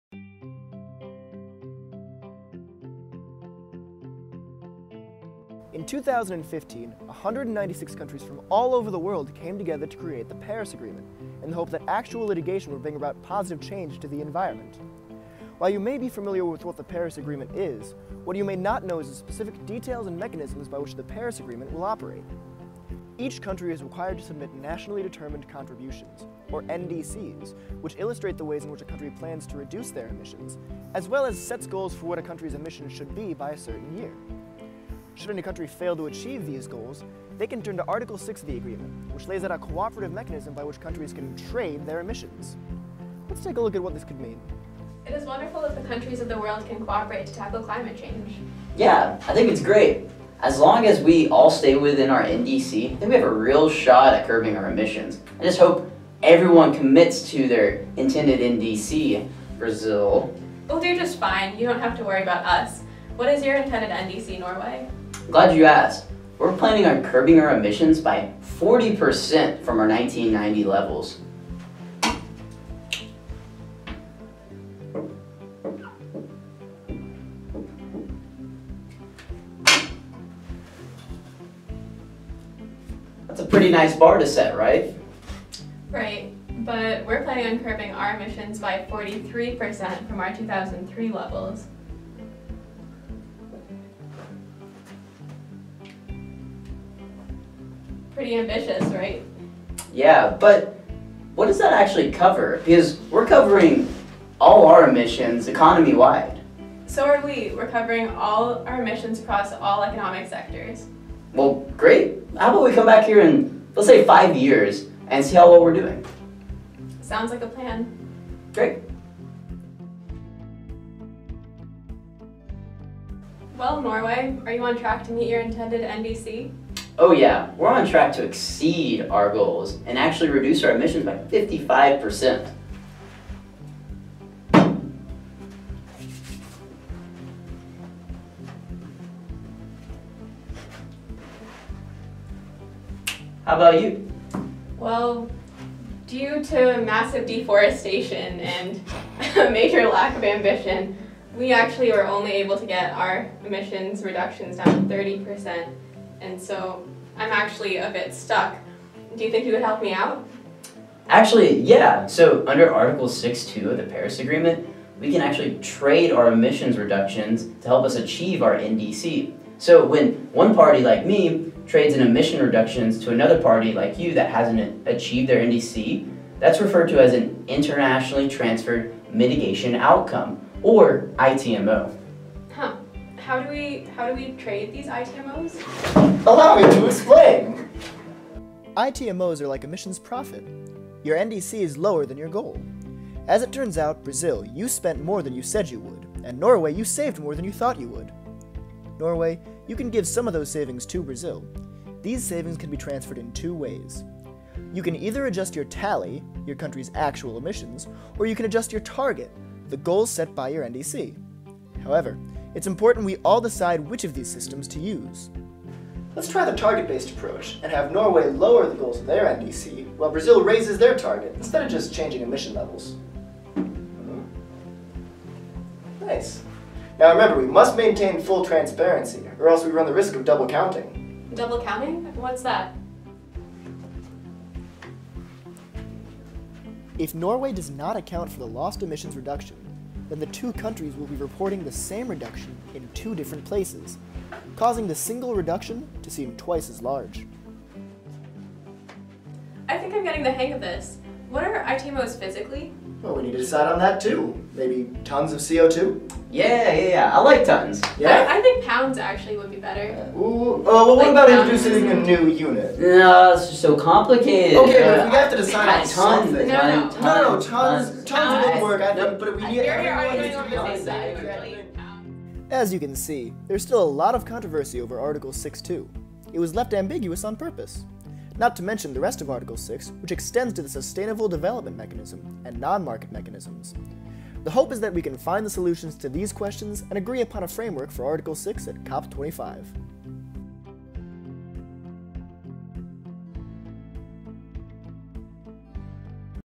Understanding Article 6
A brief and simple video explaining the basic elements of Article 6 of the Paris Agreement

Made by Lukas Palumbo, Nick Harbin, and Ambika Natarajan for Emory University EnviSci class 326/526